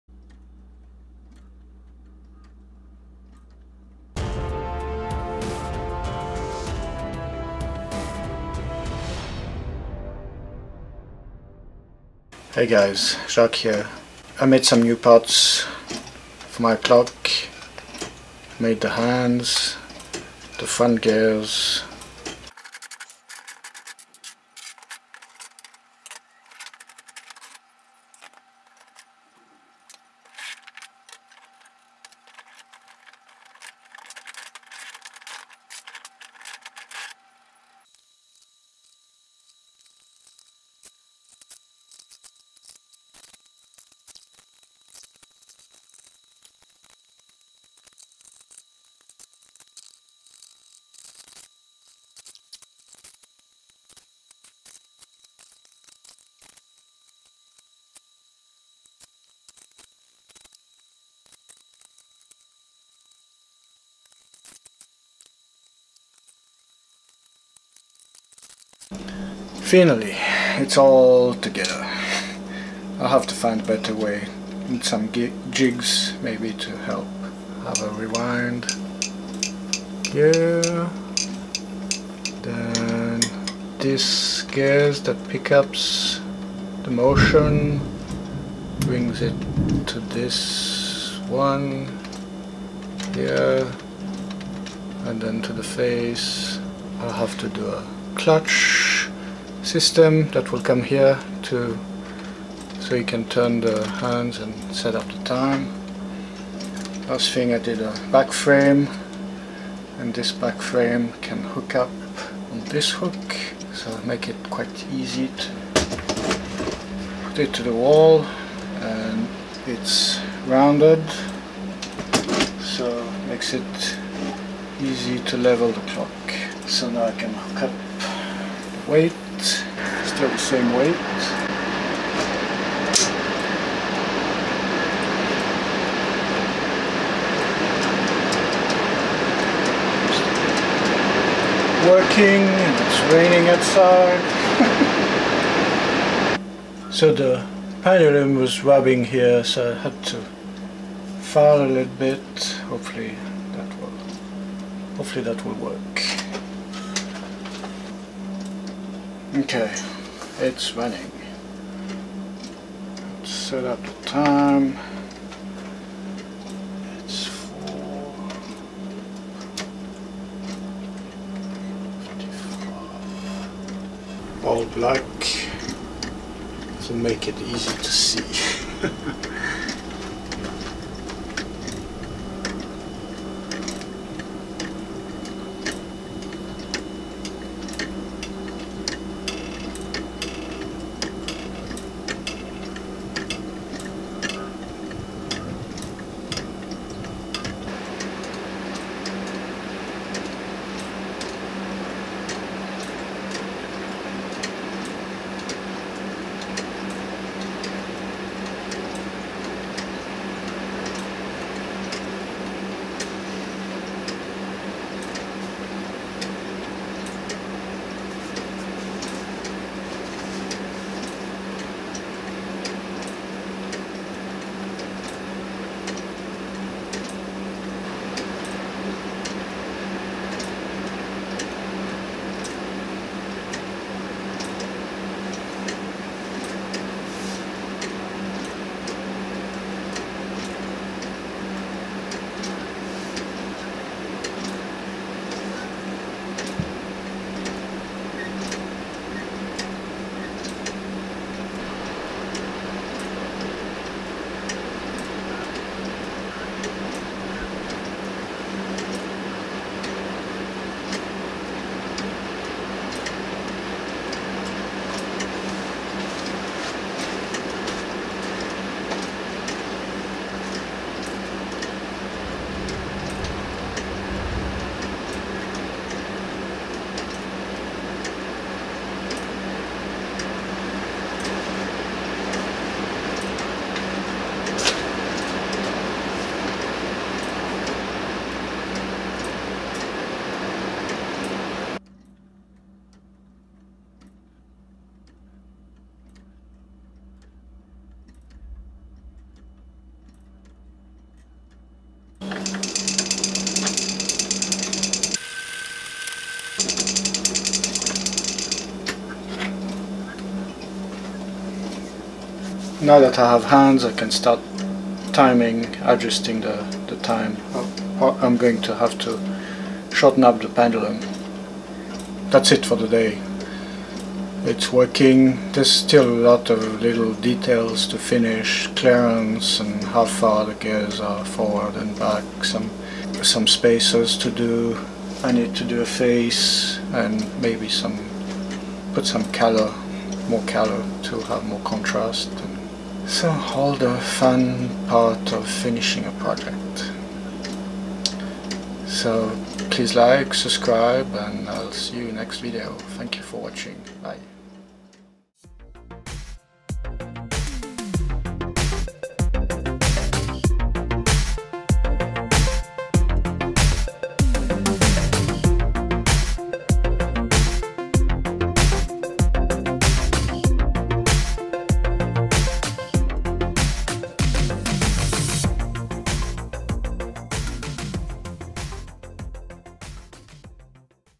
Clock Design with FreeCAD and Assembly 4 Workbench, added hands and rewind option: Part 6
I am designing a brand new 3D printed clock from scratch with FreeCAD, today I am adding the hands, a rewind gear and some updated frames
So far the clock has run all week, it has worked well
Still have to do the face, also want to do a clutch to ease the adjusting of the hands
Now I can start adjusting the length of the pendulum and see how good of a time keeper it is going to be.